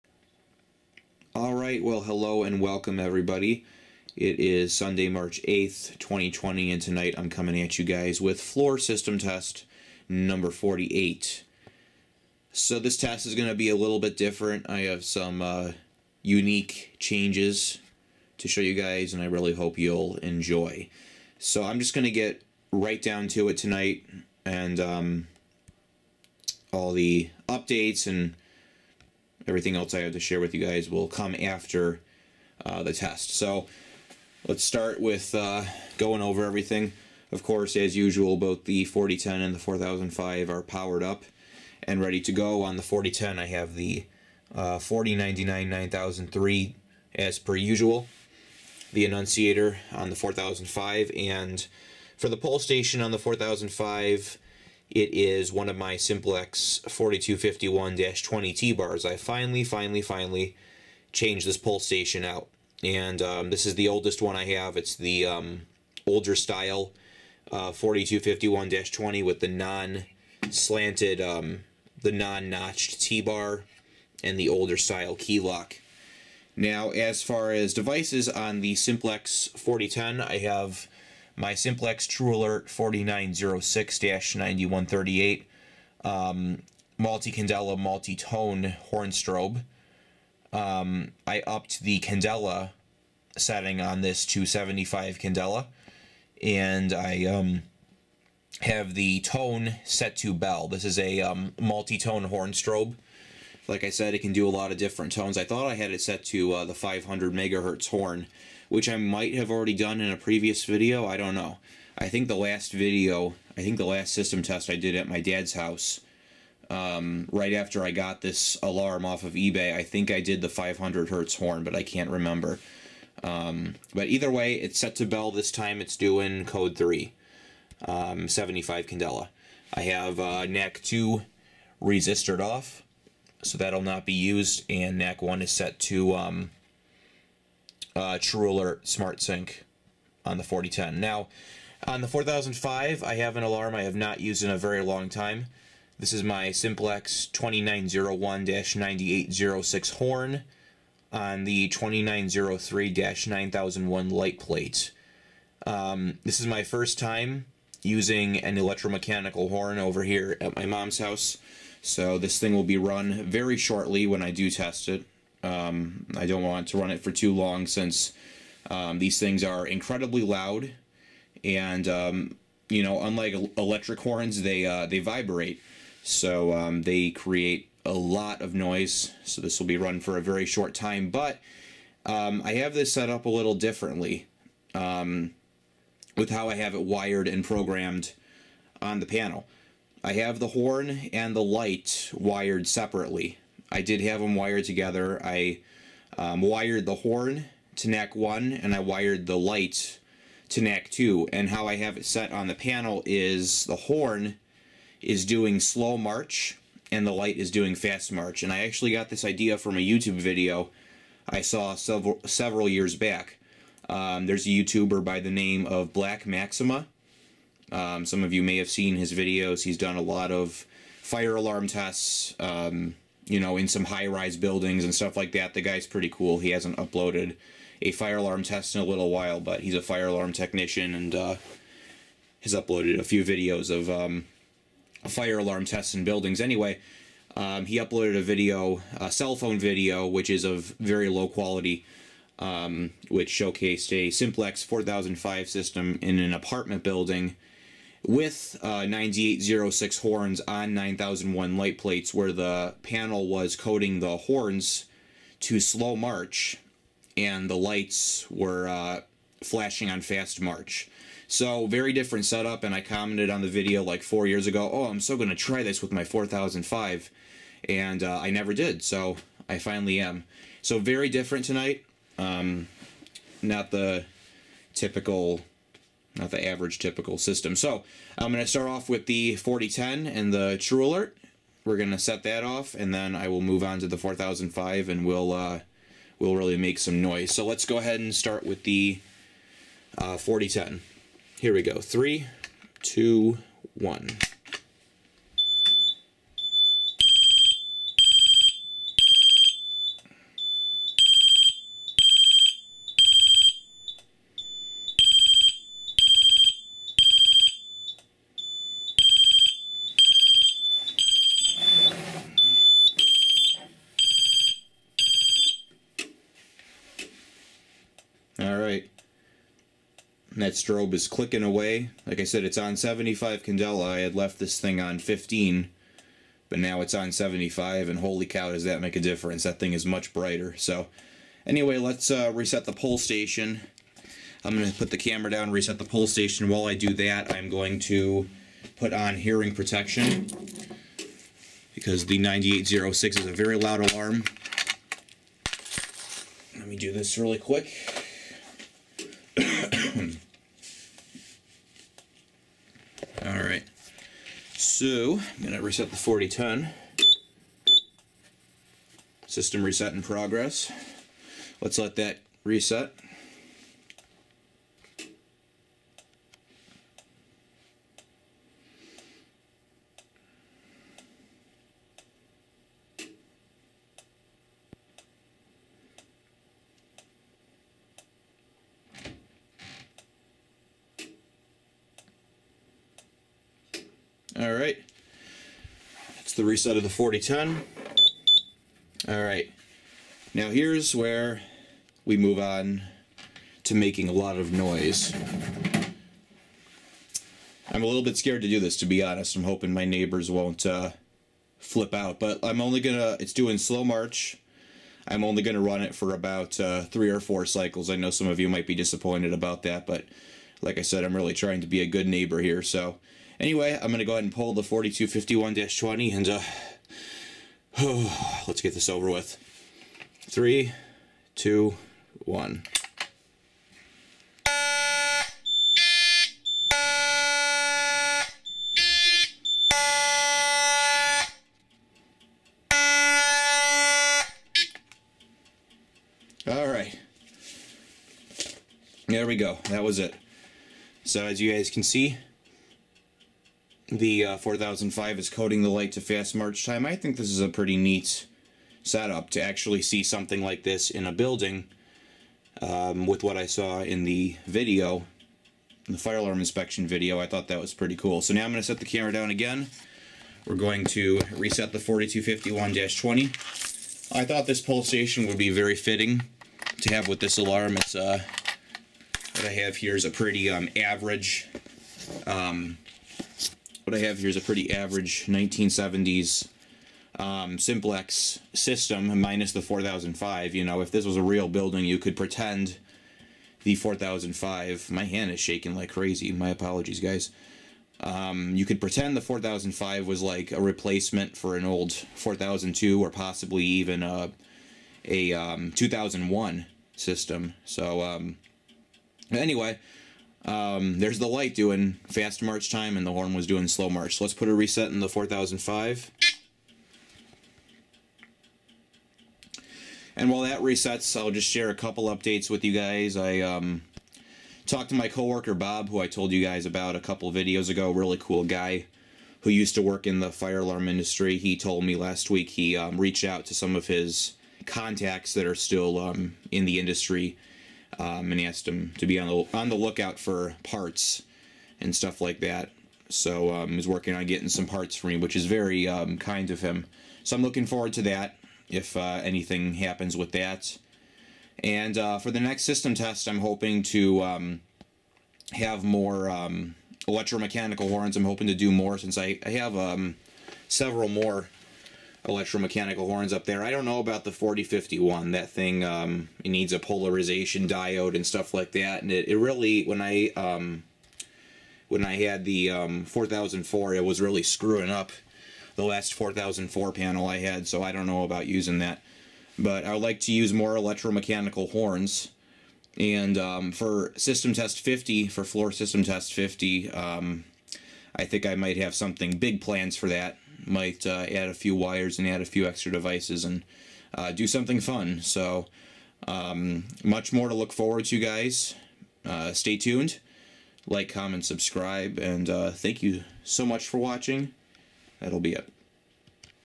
Floor System Test 48
Here is another floor system test for you guys!

Devices used in this test:

Simplex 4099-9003 addressable pull station
Simplex 4251-20 pull station
Simplex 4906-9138 TrueAlert multi-tone horn/strobe
Simplex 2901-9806 horn/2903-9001 light plate